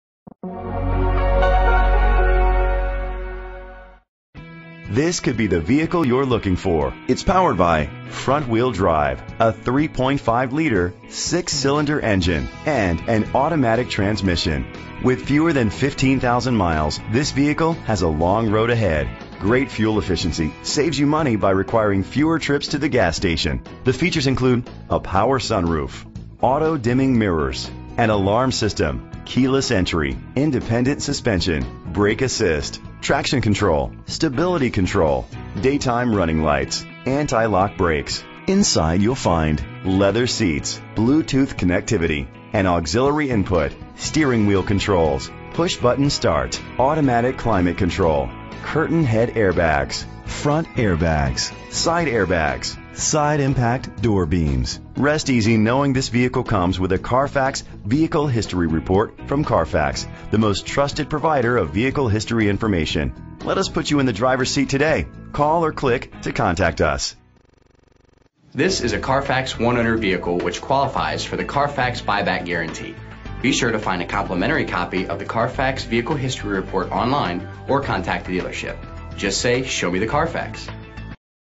2013 Lexus ES D2060458 - Englewood NJ
Year: 2013
Make: Lexus
Model: ES
Trim: 350
Engine: 3.5 liter 6 cylinder FWD
Transmission: Automatic
Color: Dk. Brown
Mileage: 10108
Address:
VIN:JTHBK1GG9D2060458